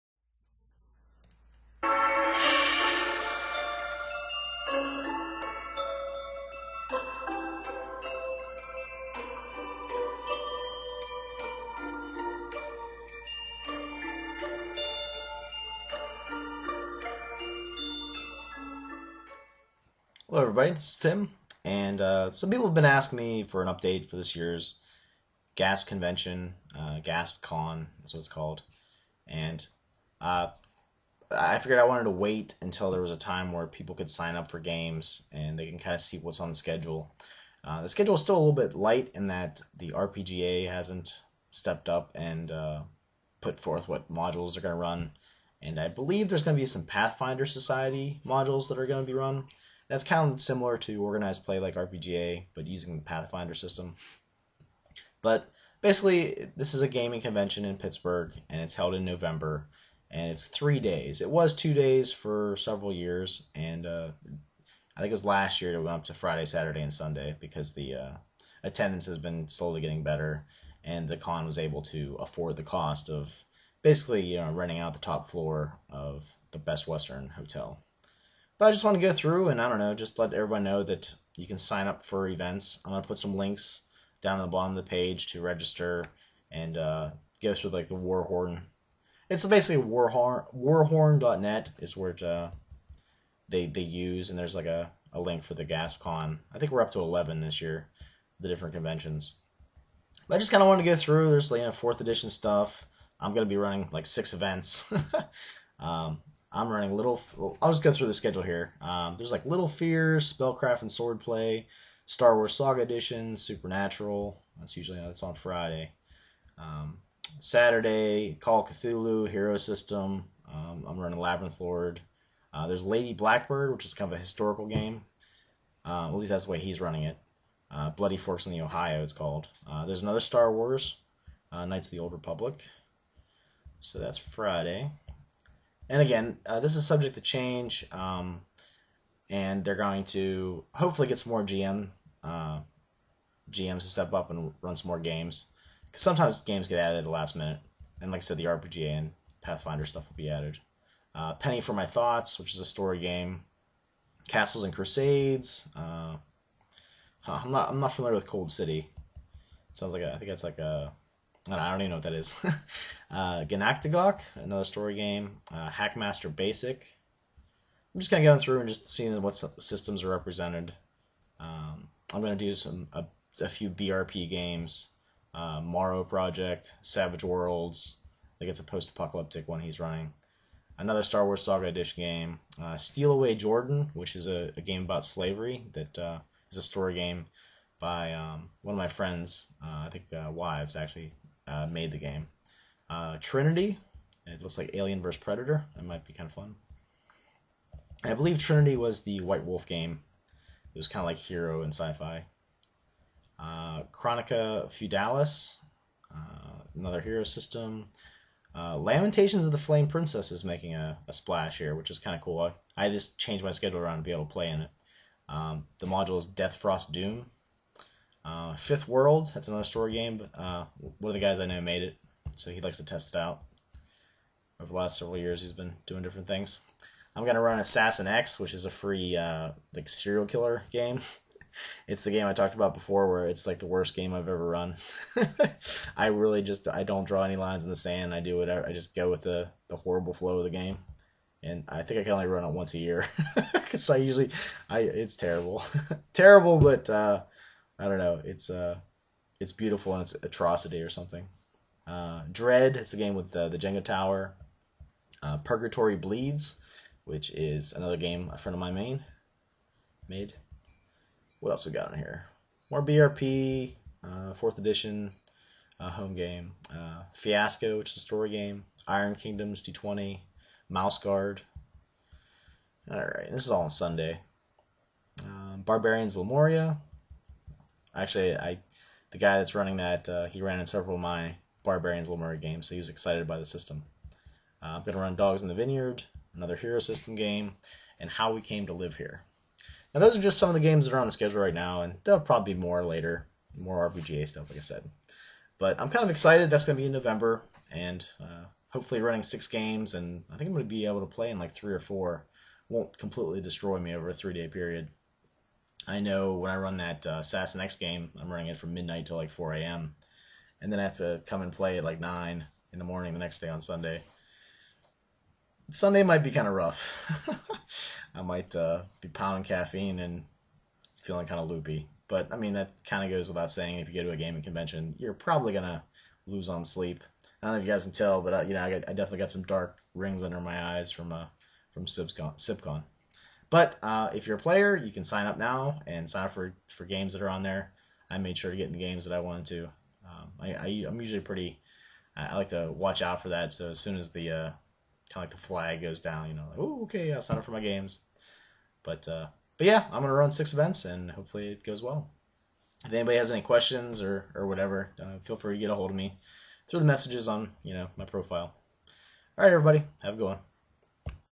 GASPCON XI 2010 Update - Player sign ups and games
There are also other events that are boardgaming, card games, and miniatures, I just mentioned the RPG events.

GASPCON XI is in Pittsburgh on Nov 12, 13, and 14 at the Best Western Hotel on the top floor.

If anyone has any questions let me know.  Thanks.